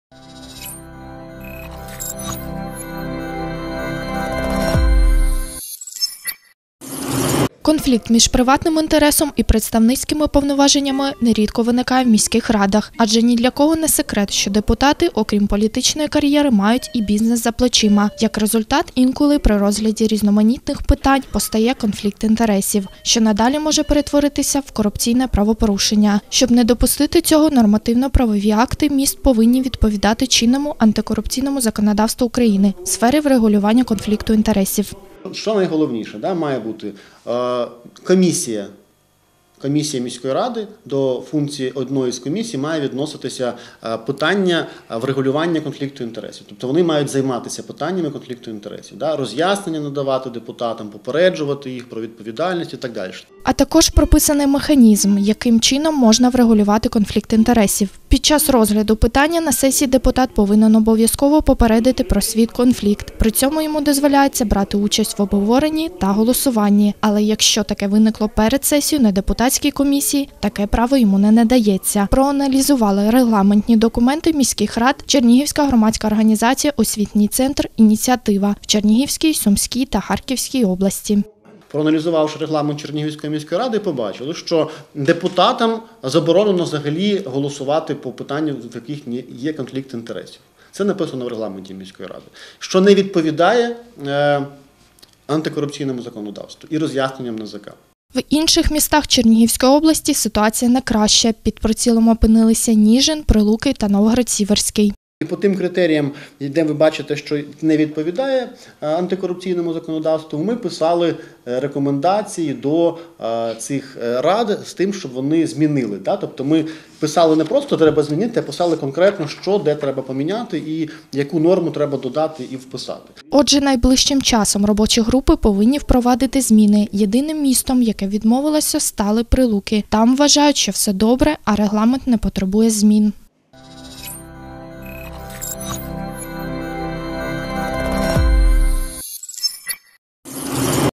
Черниговский городской совет может поменять регламент
«Образовательный центр «Инициатива» проанализировал нормативно-правовые акты городов областного значения Черниговской области.